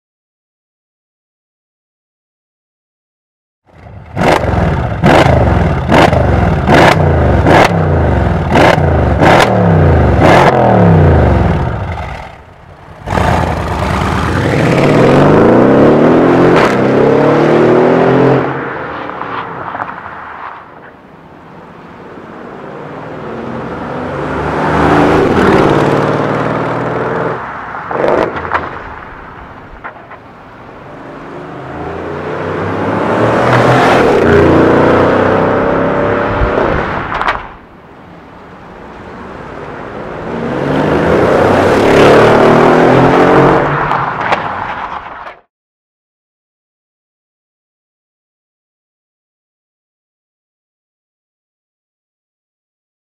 MEC Design Mercedes C217 S63 Coupé Exhaust - Apocalypse Sound Version
The MEC Design stainless steel exhaust for the Mercedes-Benz C217 S63 AMG Coupé. Apocalypse Sound Version. Hear it here now!